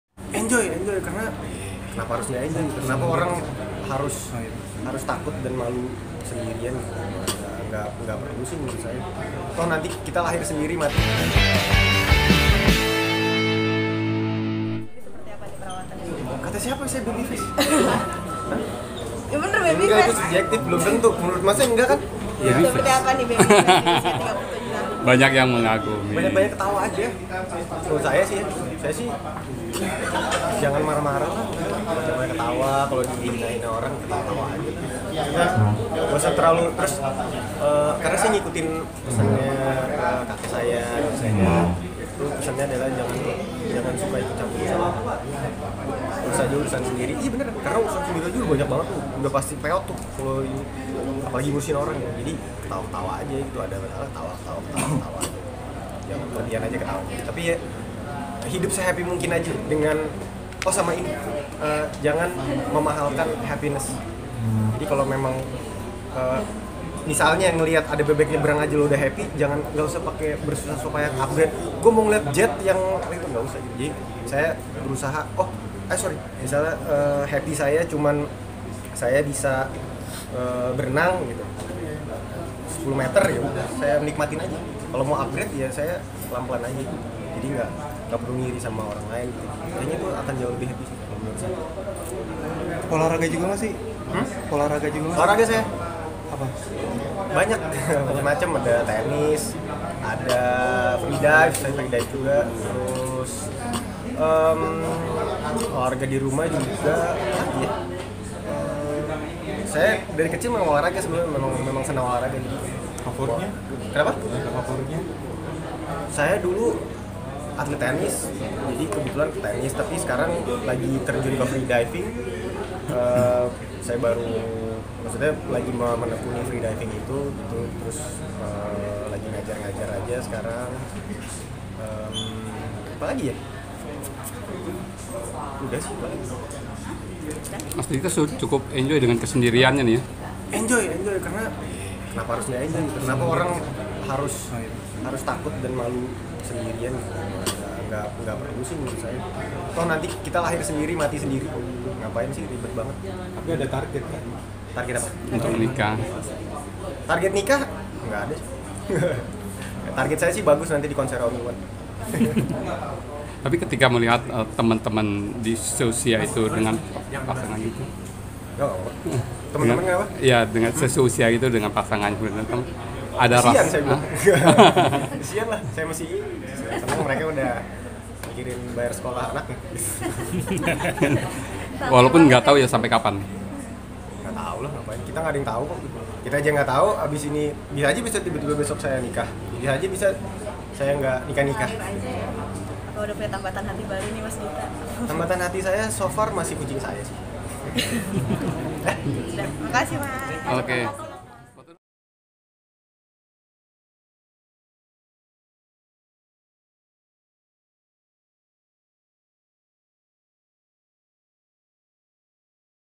DIKTA WICAKSONO TETAP ENJOY BELUM MENDAPATKAN PENDAMPING
Dikta wicaksono tetap enjoy dengan kesendiriannya